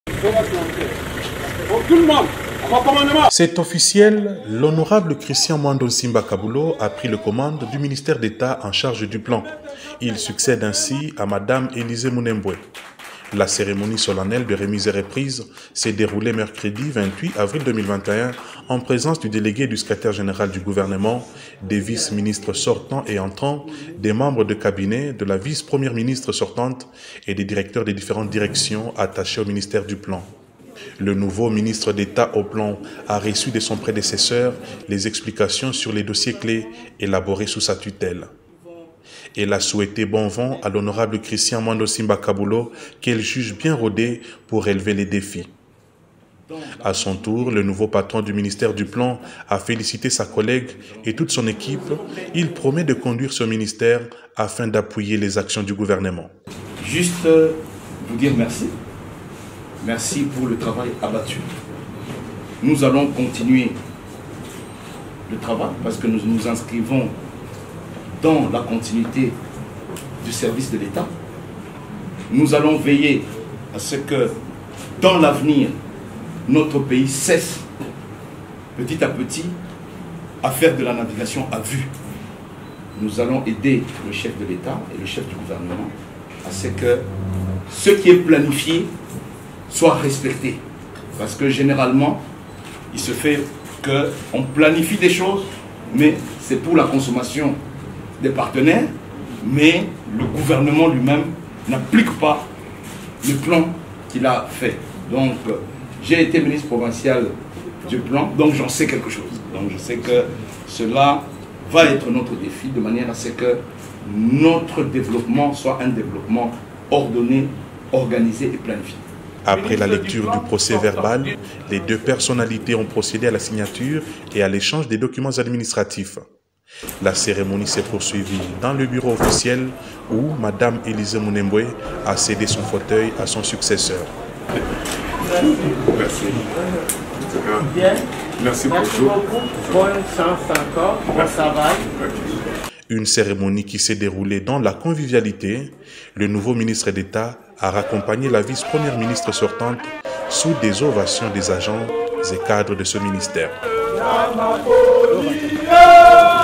Politique: Christian Mwando prend officiellement la commande du ministère du plan.
Vidéo de Vallée Lukusa